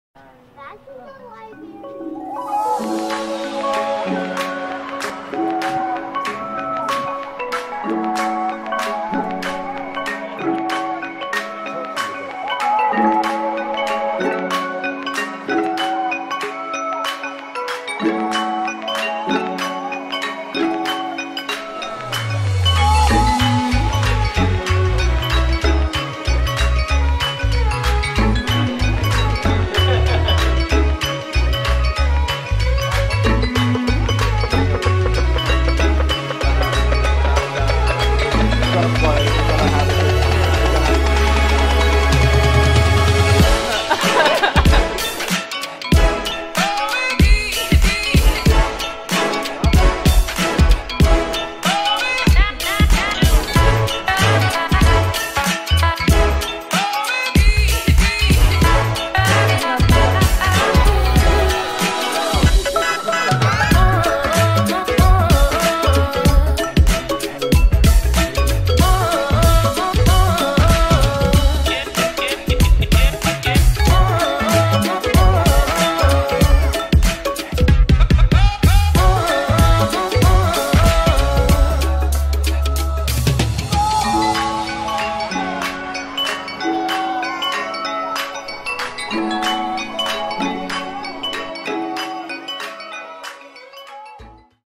Germantown Friends School Open House: 2016-17
Experience a GFS Open House visit. Video created by Lilly Dupuis '17.